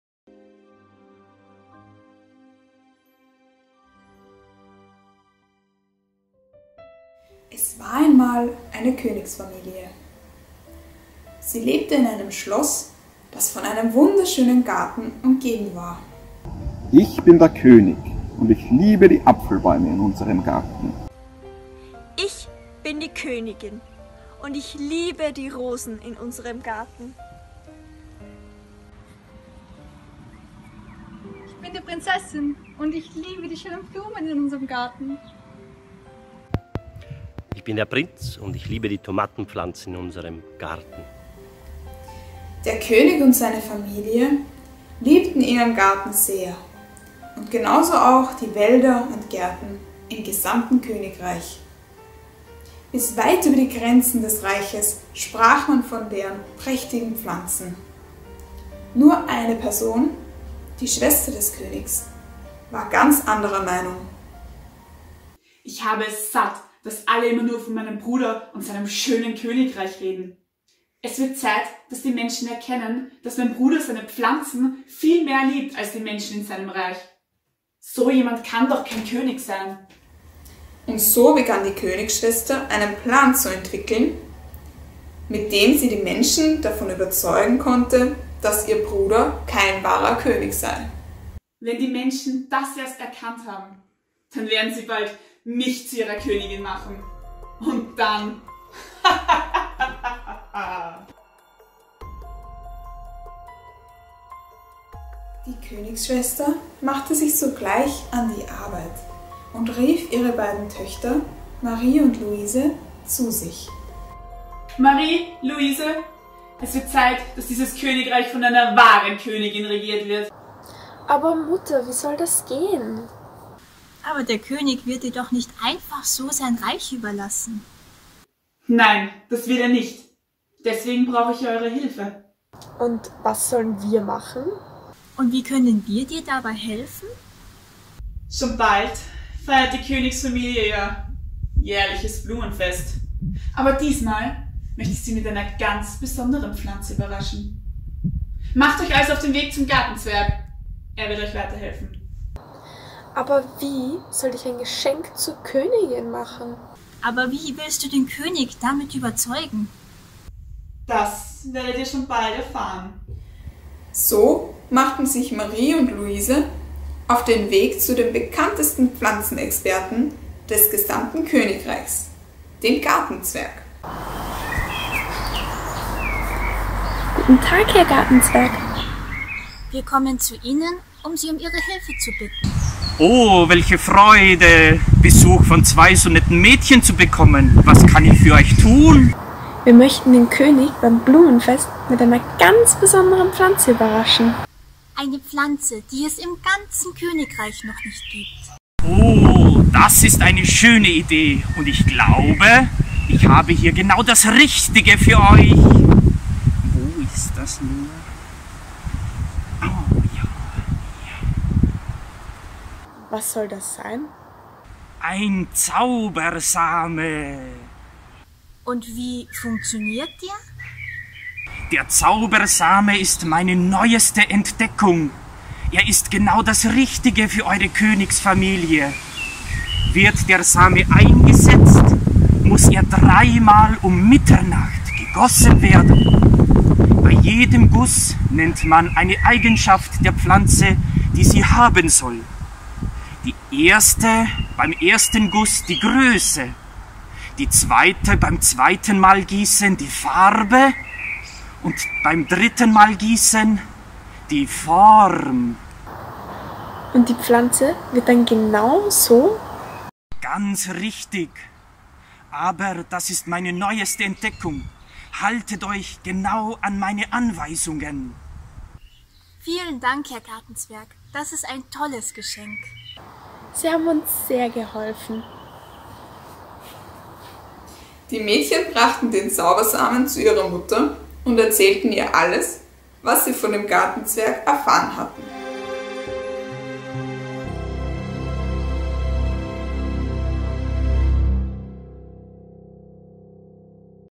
Art 4 Future - Des Königs neue Pflanze | Act Now (ESK Projekt)
Ein Umweltmärchen von und mit dem Act Now Umwelttheater.

Ein europäisches Solidaritätsprojekt gefördert durch das Europäische Solidaritätskorps
Unterstützt von der Jugendpastoral der Salesianer Don Boscos Österreich